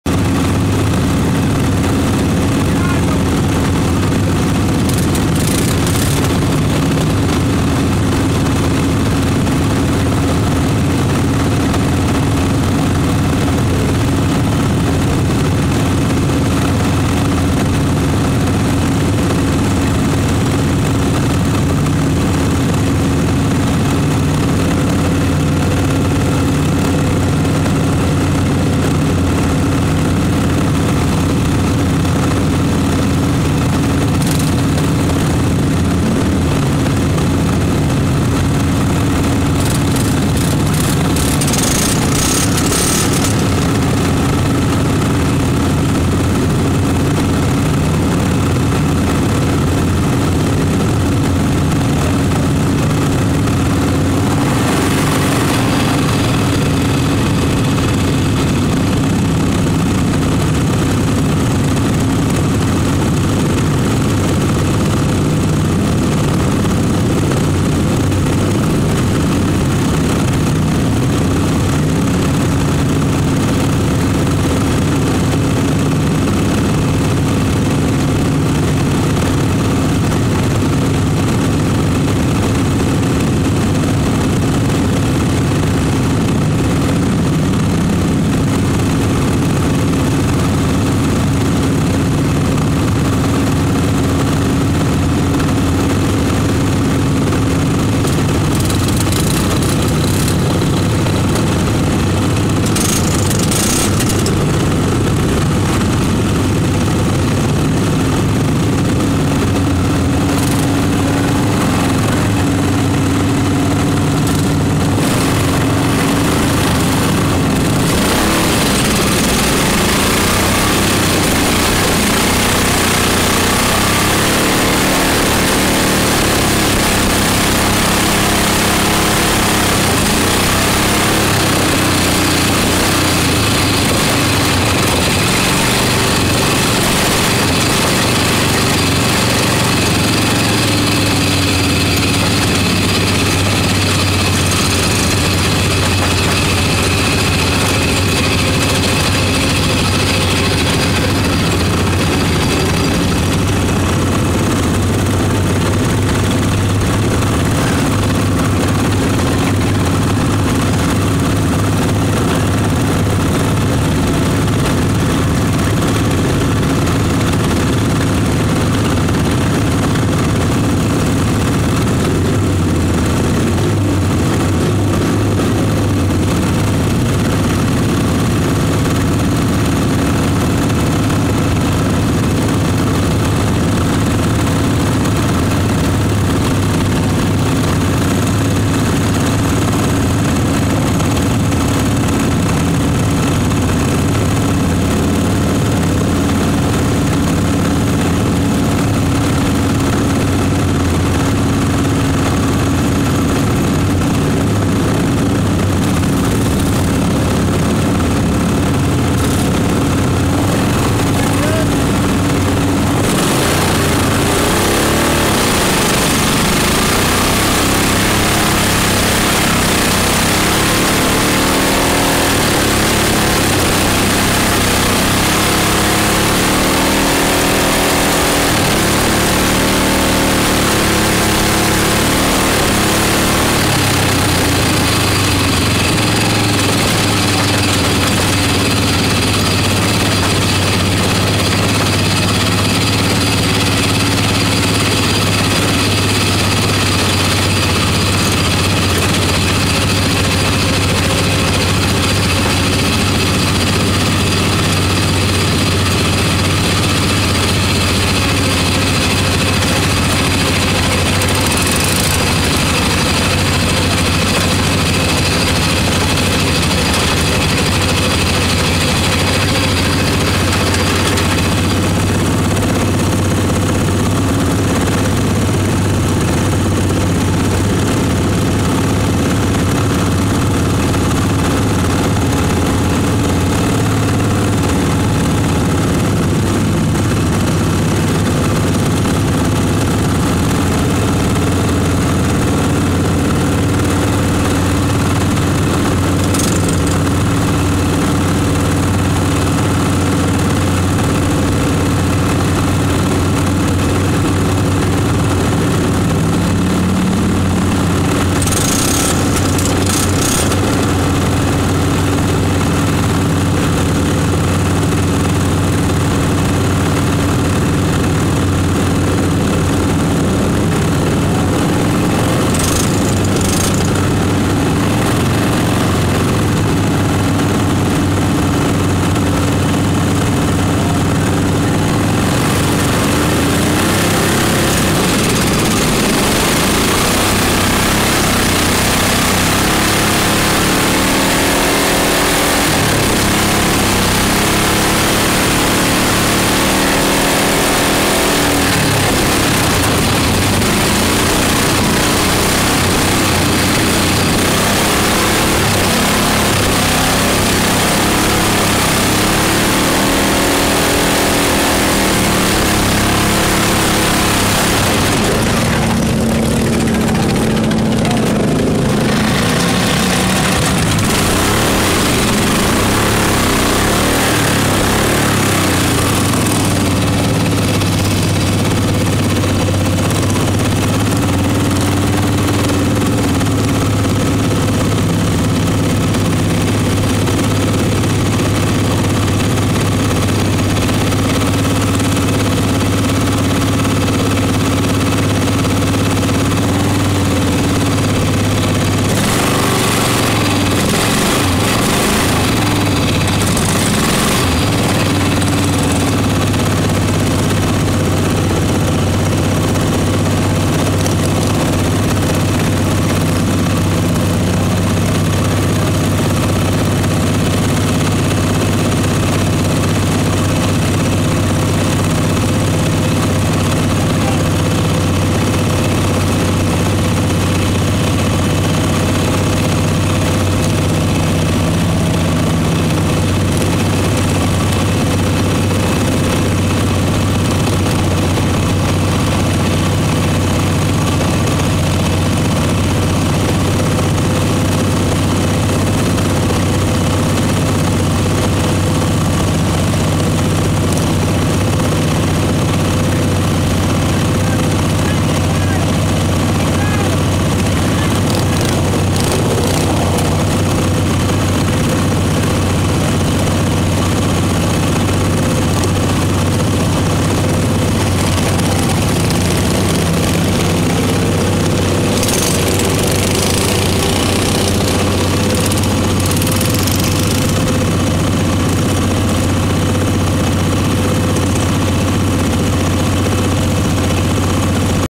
2013 northwestern mi fair lawn mower feature
Lawnmower cam on #99, spindle broke after a few laps while leading